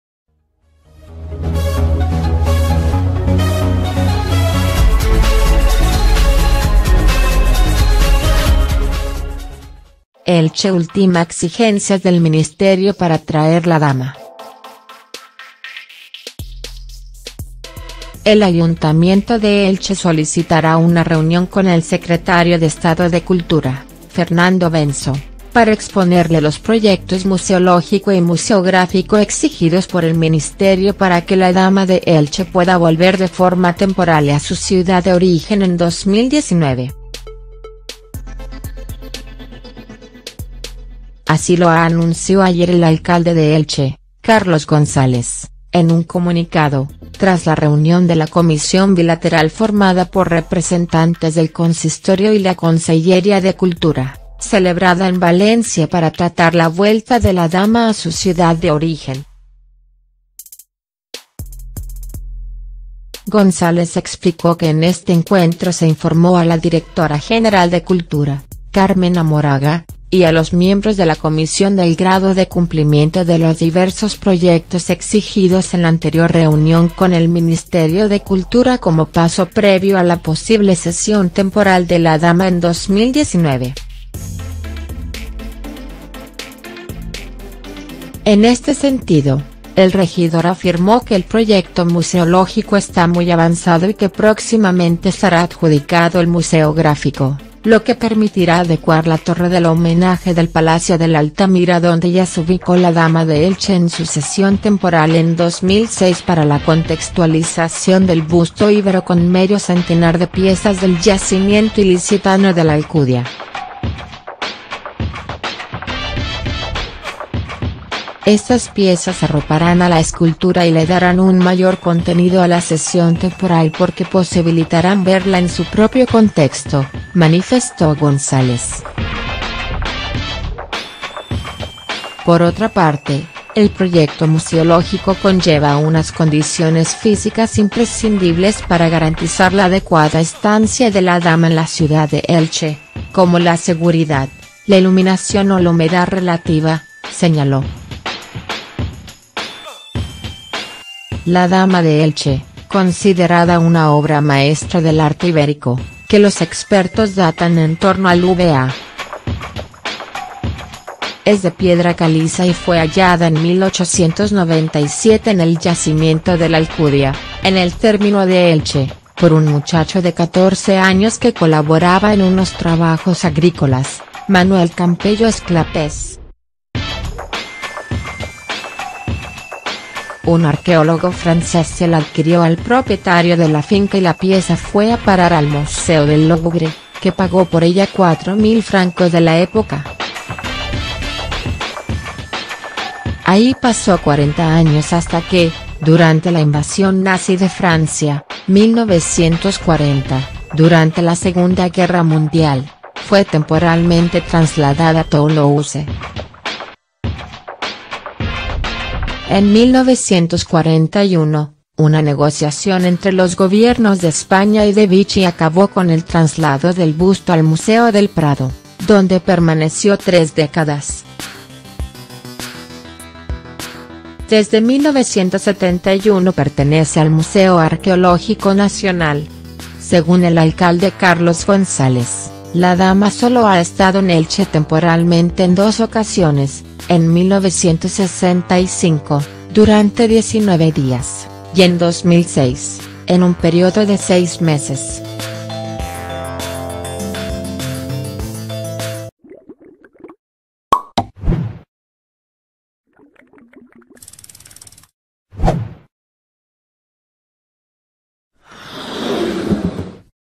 Elche ultima exigencias del Ministerio para traer la Dama
Últimas noticias: Elche ultima exigencias del Ministerio para traer la Dama